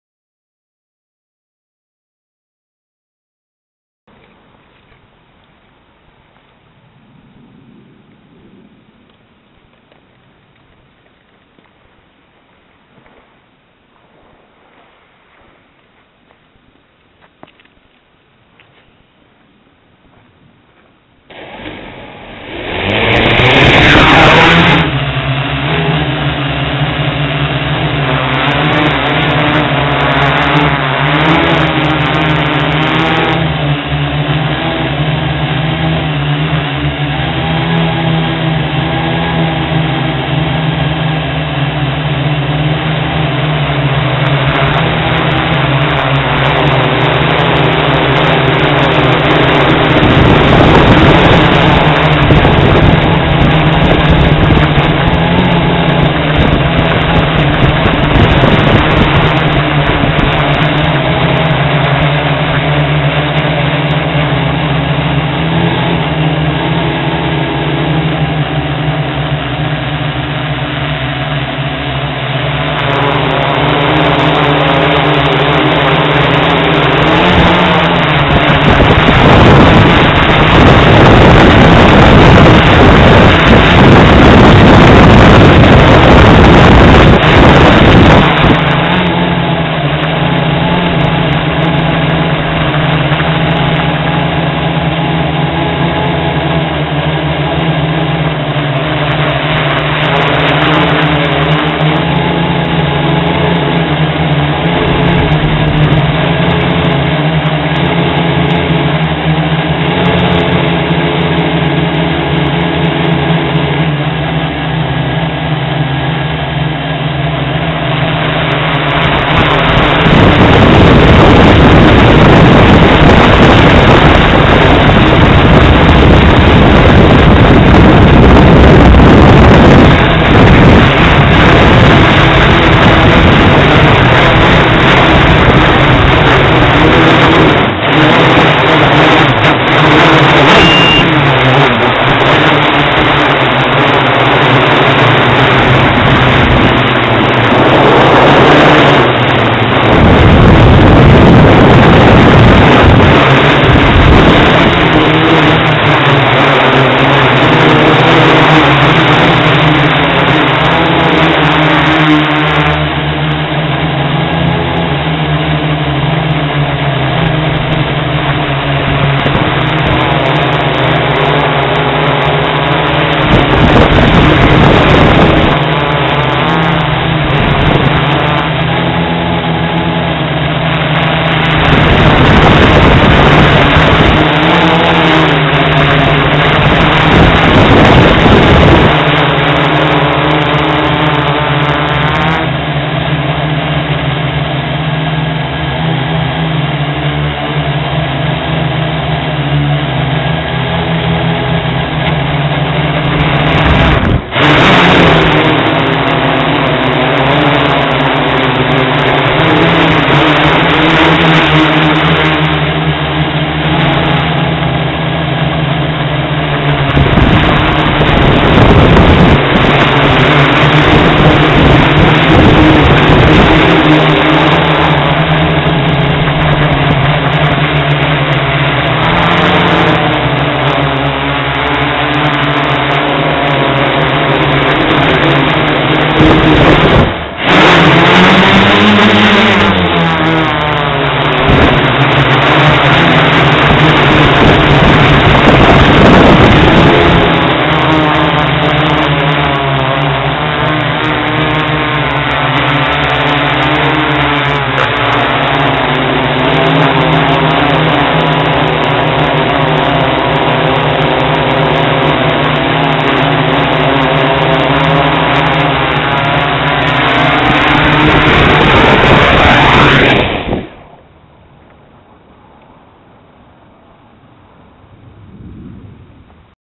6th flight West Beach on whidbey
HK wing camera facing aft. 6Feb14 short flight with the multicopter it was too cold!